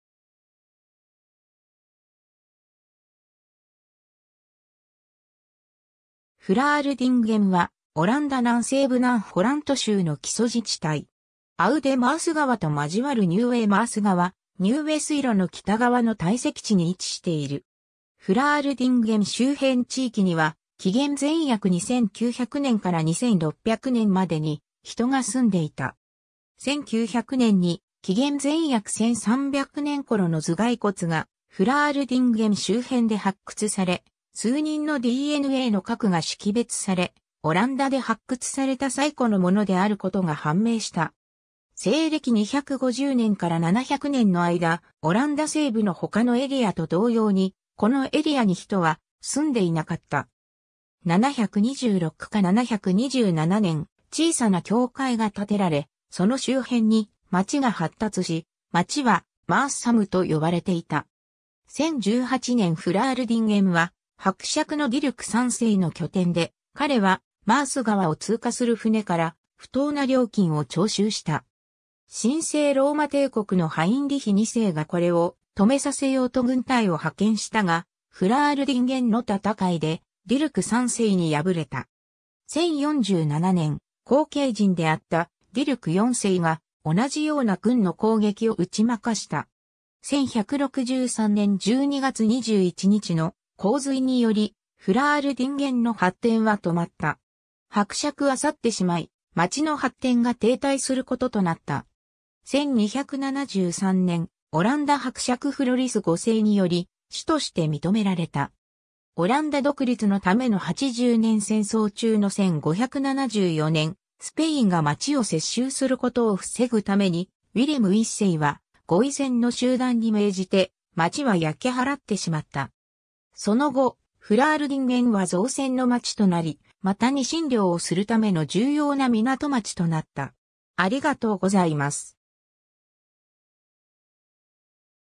フラールディンゲン
フラールディンゲン（蘭: Vlaardingen [ˈvlaːrdɪŋə(n)] ( 音声ファイル)）は、オランダ南西部南ホラント州の基礎自治体（ヘメーンテ）。
 アウデ・マース川と交わるニューウェ・マース川/ニューウェ水路 の北側の堆積地に位置している。
 フラールディンゲン周辺地域には、紀元前約2900年から2600年までに、人が住んでいた。
1900年に、紀元前約1300年頃の頭蓋骨が、フラールディンゲン周辺で発掘され、数人のDNAの核が識別され、オランダで発掘された最古のものであることが判明した。
 西暦250年から700年の間、オランダ西部の他のエリアと同様に、このエリアに人は住んでいなかった。
 726か727年、小さな教会が建てられ、その周辺に街が発達し、街は"Marsum"と呼ばれていた。
 1018年フラールディンゲンは、伯爵のディルク3世の拠点で、彼はマース川を通過する船から不当な料金を徴収した。
神聖ローマ帝国のハインリヒ2世がこれを止めさせようと軍隊を派遣したが、フラールディンゲンの戦いで、ディルク3世に敗れた。
1047年、後継人であったディルク4世が、同じような軍の攻撃を打ち負かした。
 1163年12月21日の洪水により、フラールディンゲンの発展は止まった。
伯爵は去ってしまい、街の発展が停滞することとなった。
 1273年、オランダ伯爵フロリス5世により、市 (シティ) として認められた。
 オランダ独立のための八十年戦争中の1574年、スペインが街を接収することを防ぐために、ウィレム1世は、ゴイセンの集団に命じて、街は焼き払ってしまった。
その後、フラールディンゲンは造船の街となり、またニシン漁をするための重要な港町となった。